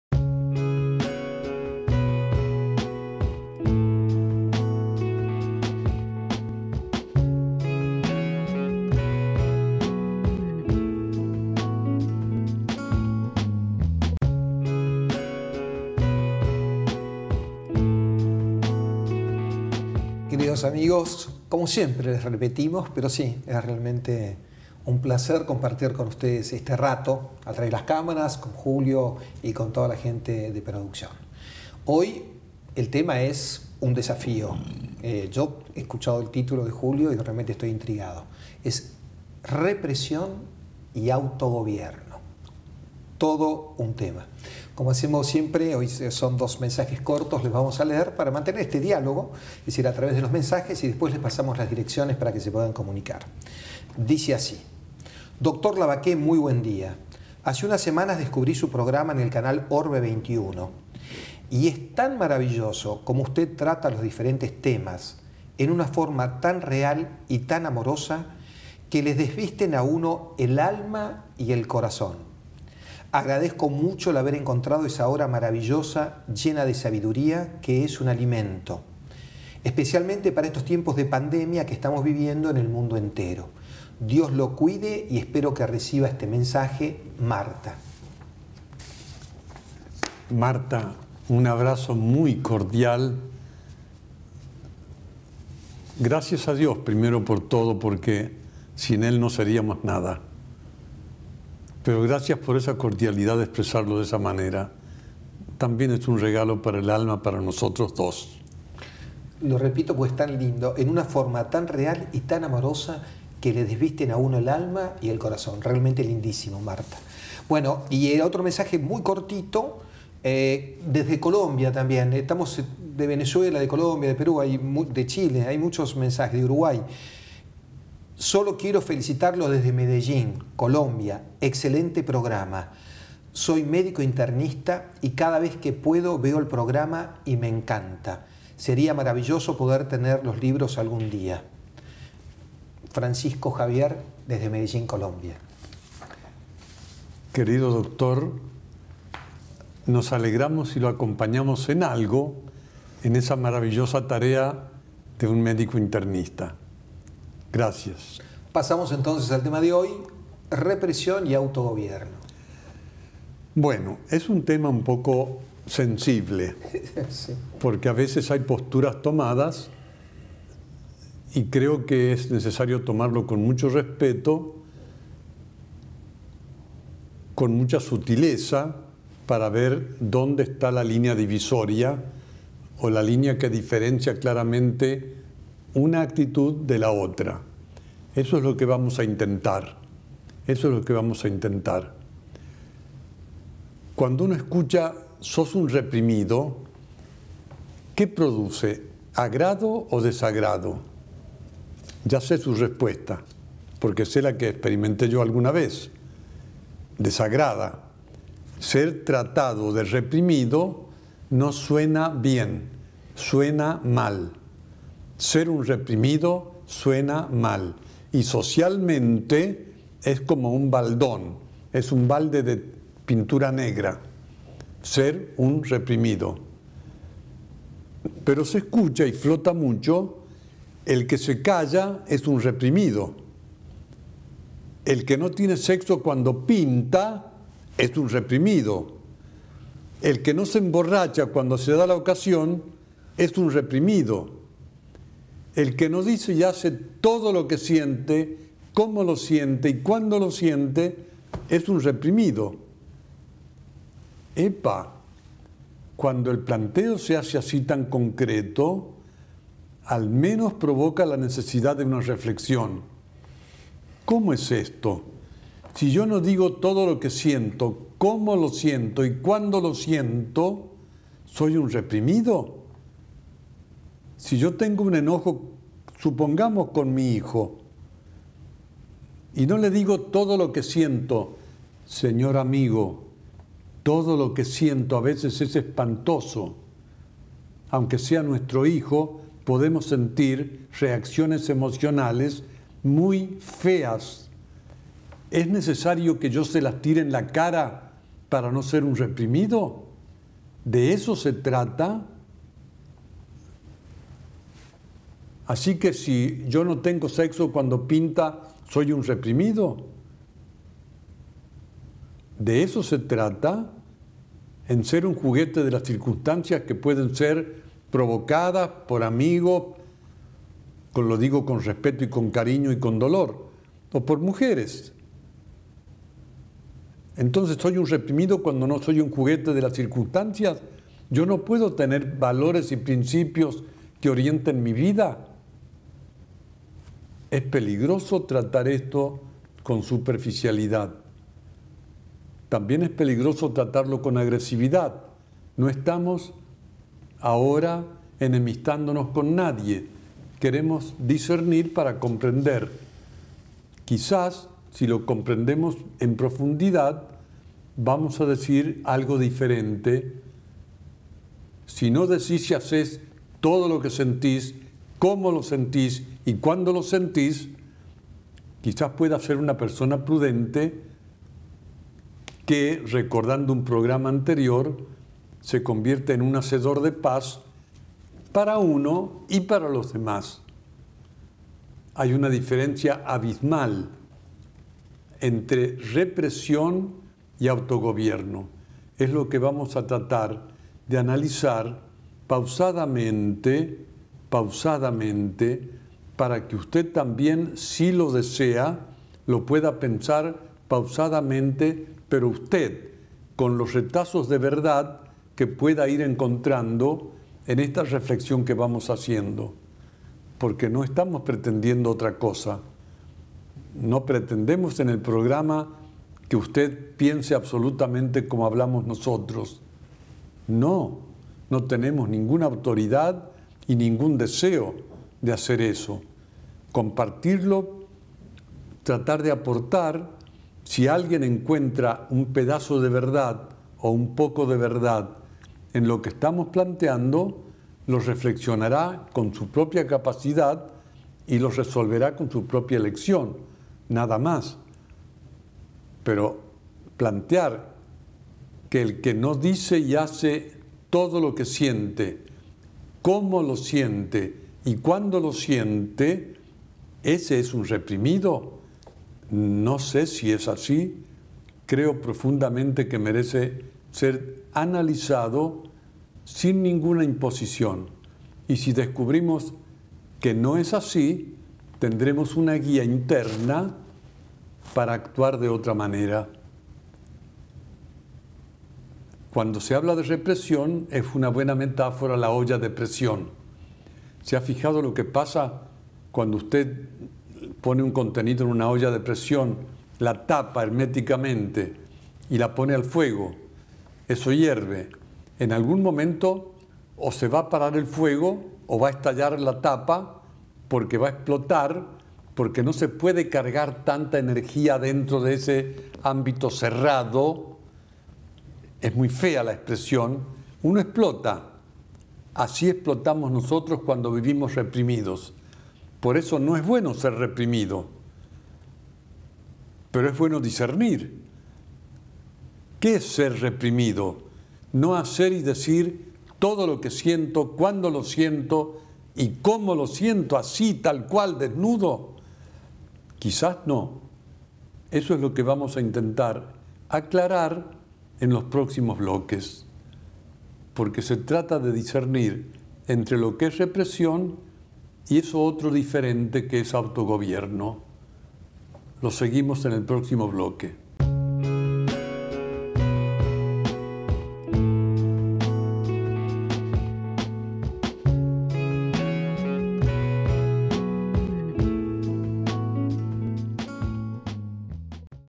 Diálogos con la vida: Represión y autogobierno
Compartimos un fragmento de DiálogosConLaVida donde reflexionamos junto al Dr. Julio César Labaké sobre la represión y el autogobierno.